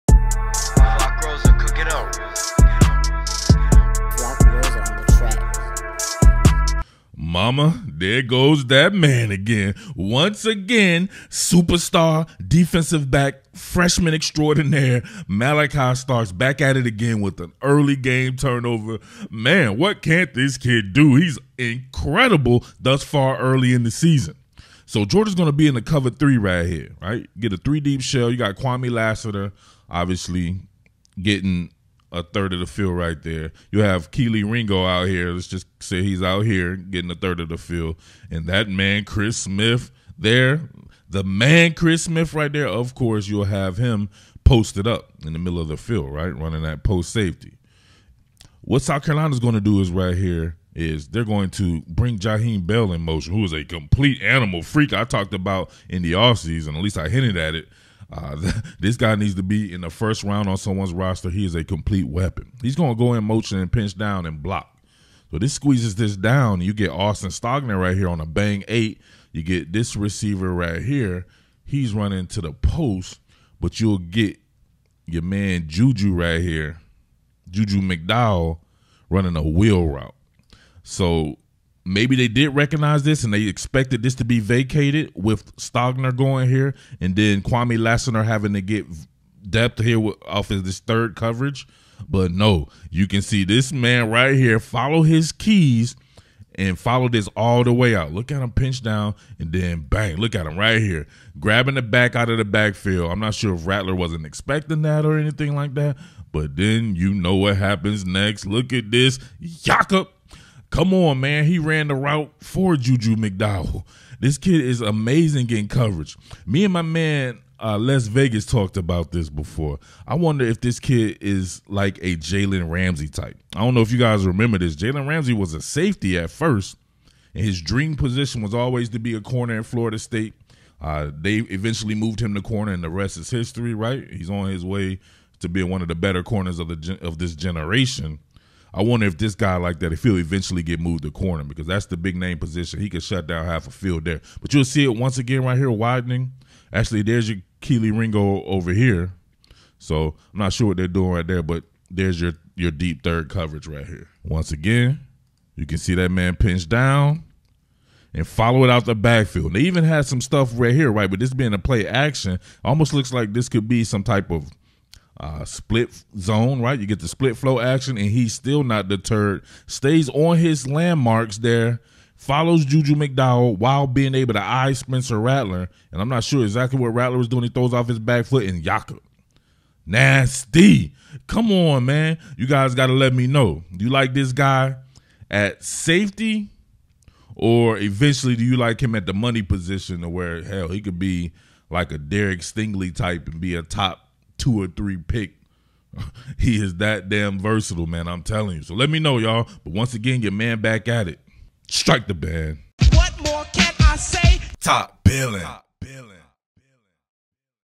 Mama, THERE GOES THAT MAN! UGA freshman Malaki Starks back at it!!
Top Billin' dissects another heady play by Georgia Bulldogs super freshman DB Malaki Starks who is looking like the GOODS!